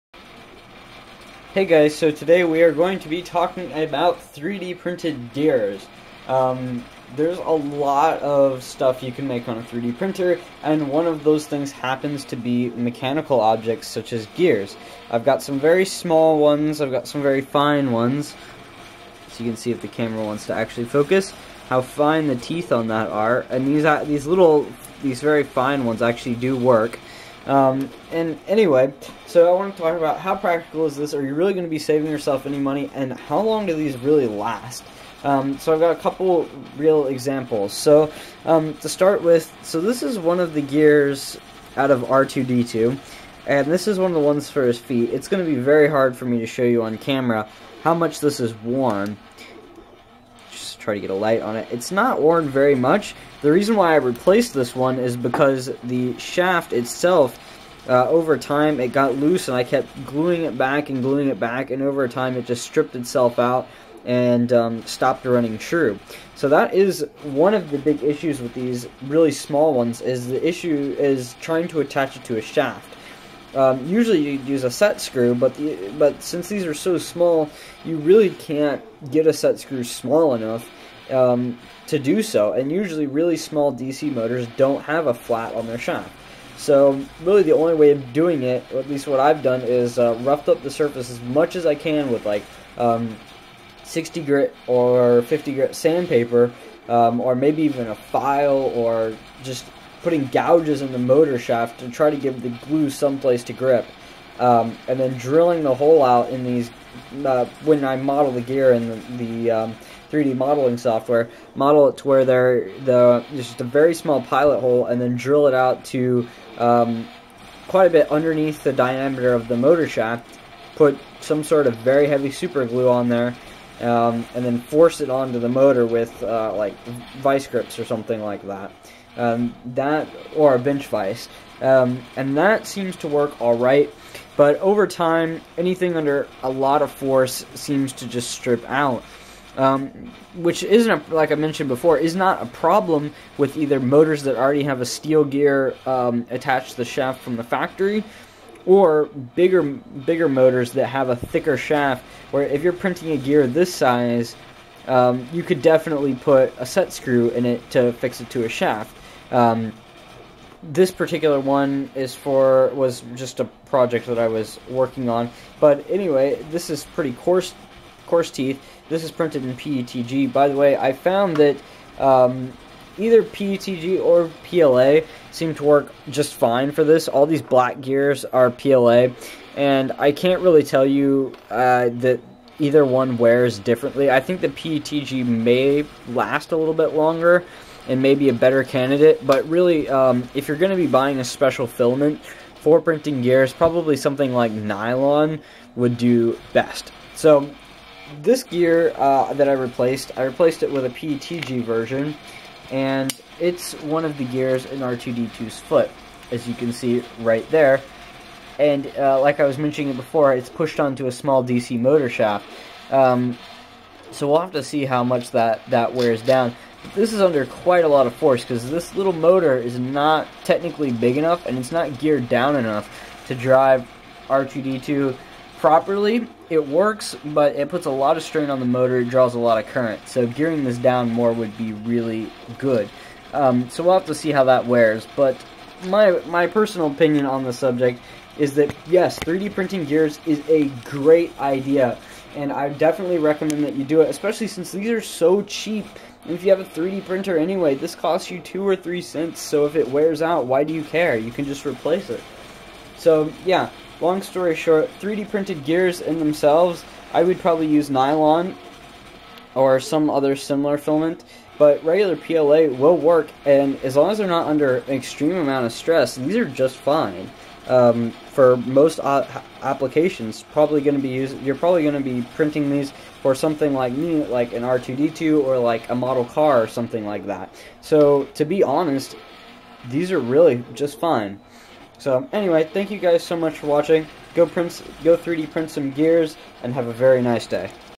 Can you actually use 3D printed gears?
Today I will be talking a bit about 3D printing gears for mechanical applications such as RC cars or robotics. Hope you enjoy!